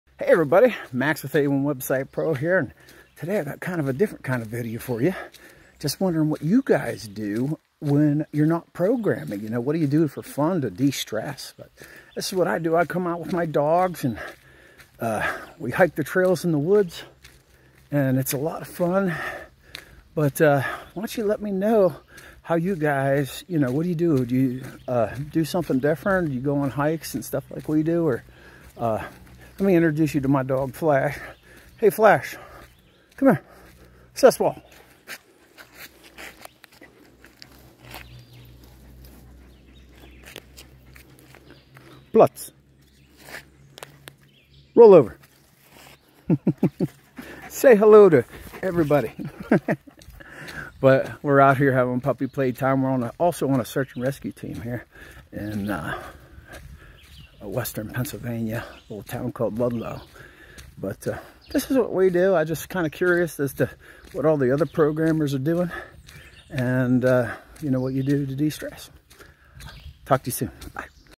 what do you guys do when you're not programming?
just wondering what everybody else does when they're not programming. like what do you do for fun after you step away from that computer?